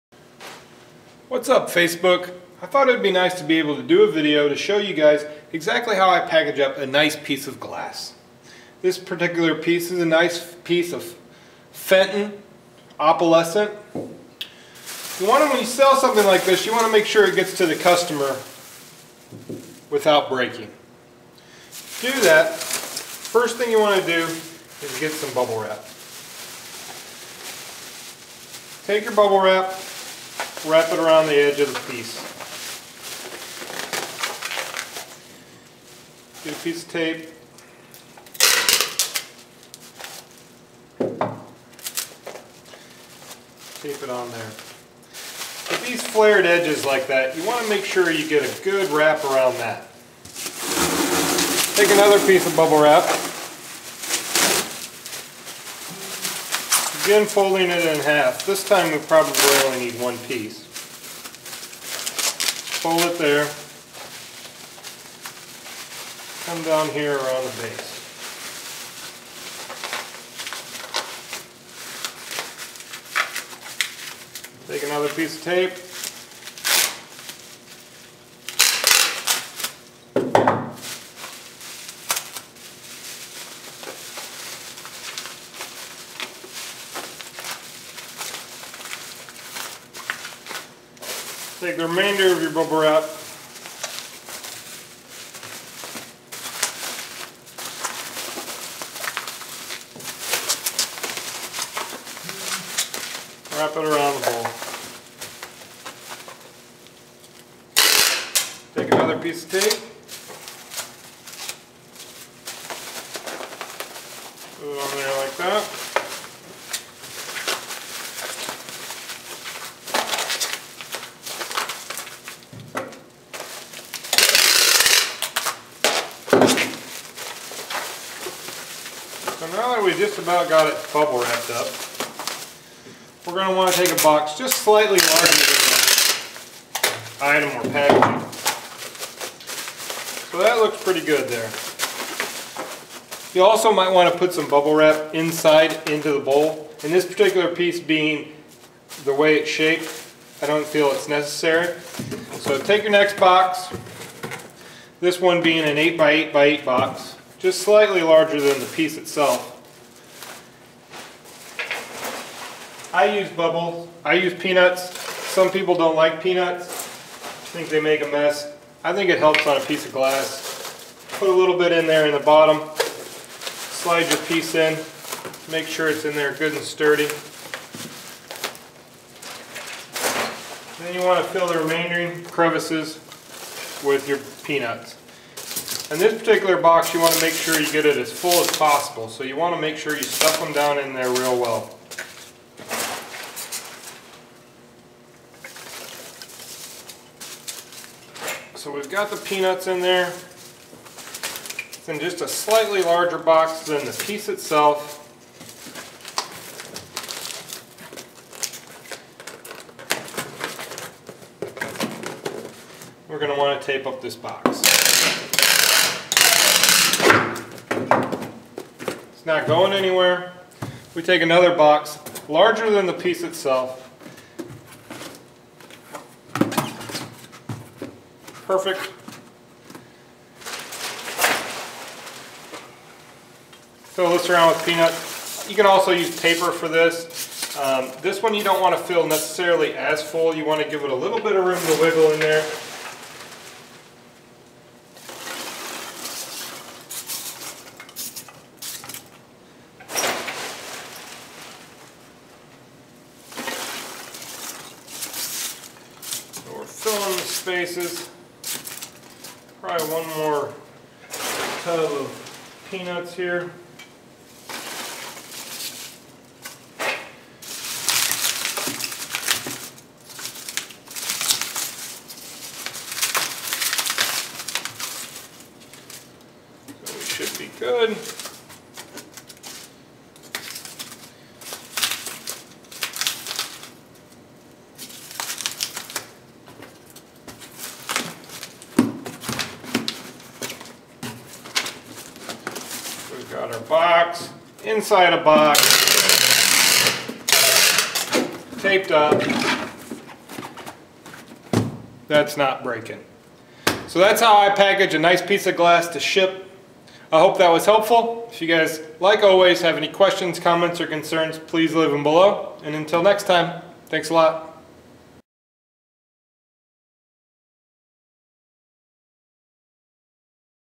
How to Package Glass! Shipping and Selling on Ebay.
Getting your product to the customer is the most important step when selling on eBay.  Taking the time to package your product correctly not only makes the customer happy it also cuts down on returns and refunds.  Keeping a 5 star rating will save you money!!